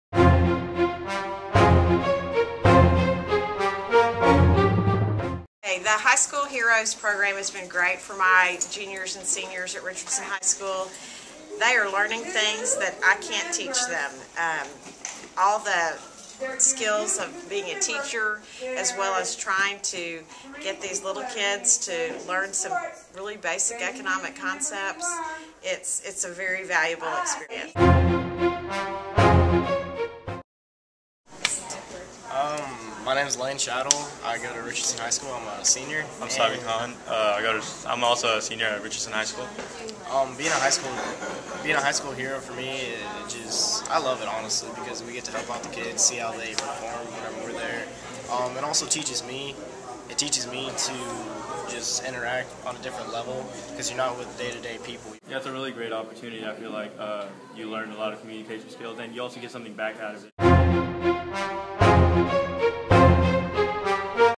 JA High School Heroes
Richardson High School students taught JA curriculum through the High School Heroes program to students at Northwood Elementary in Richardson ISD.